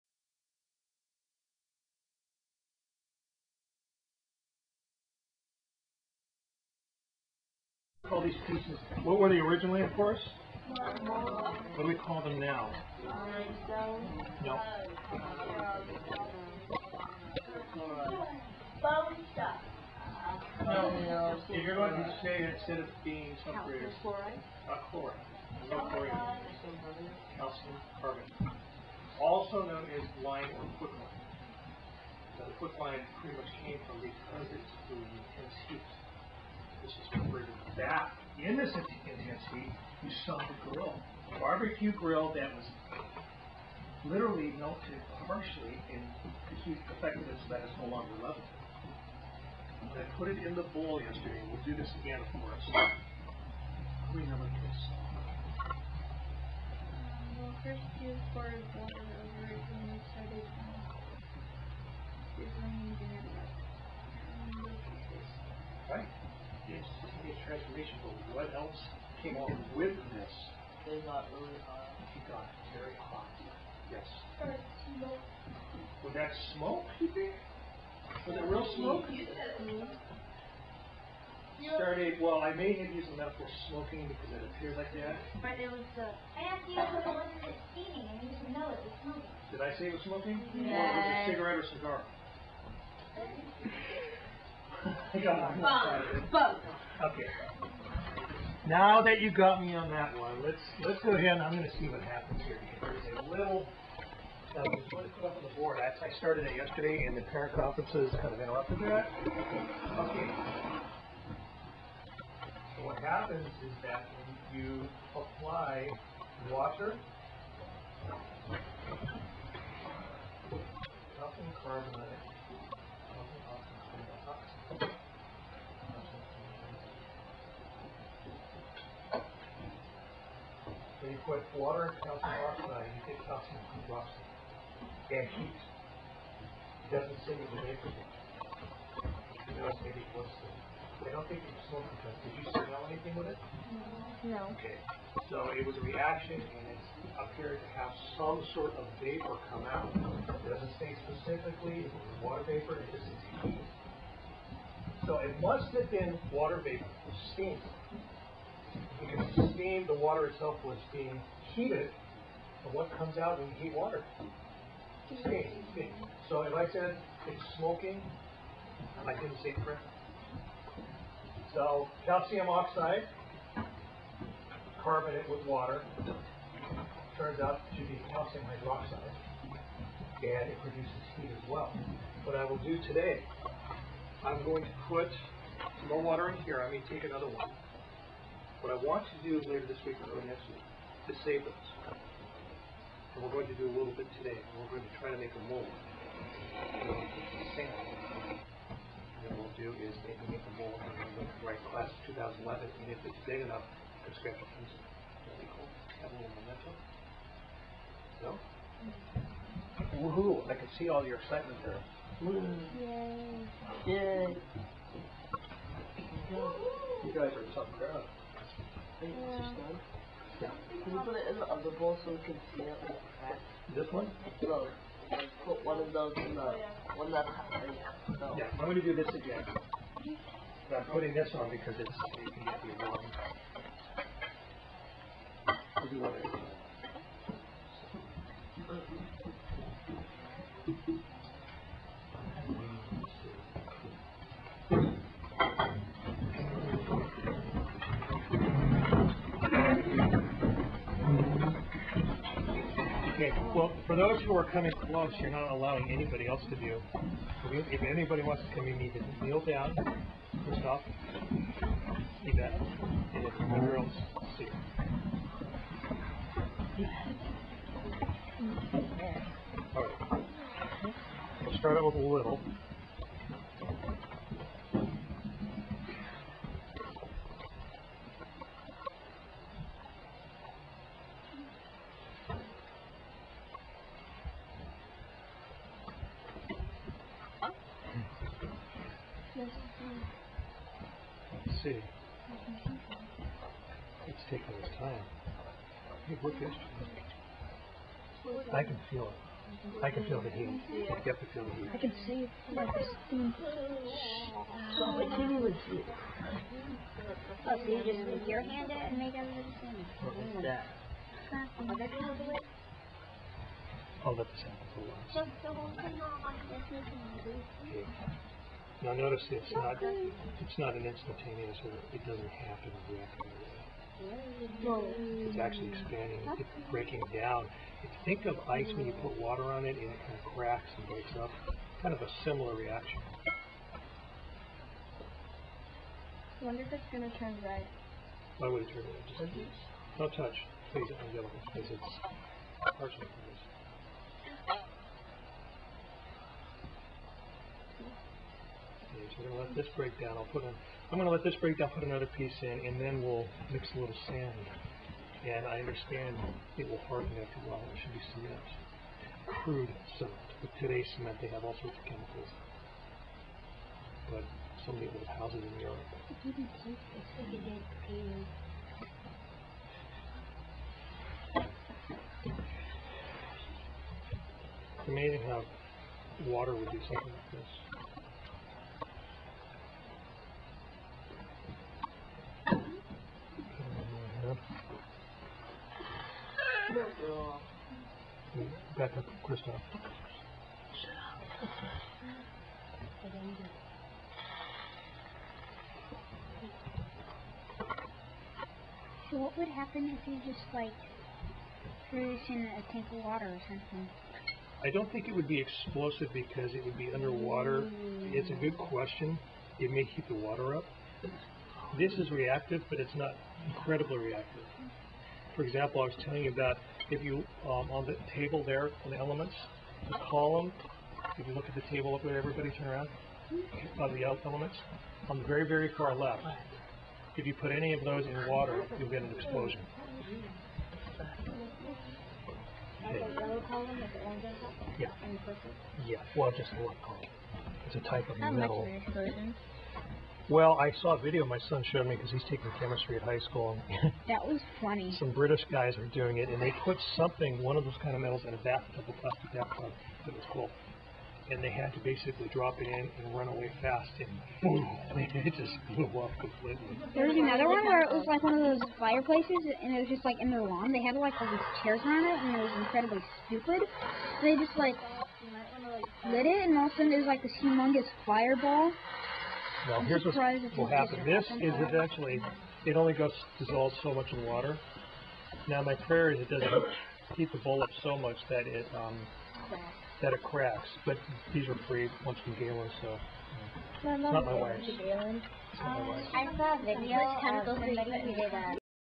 Marble (Calcium Carbonate) and water day 2 part 1/2
this is day two of the continued marble experiment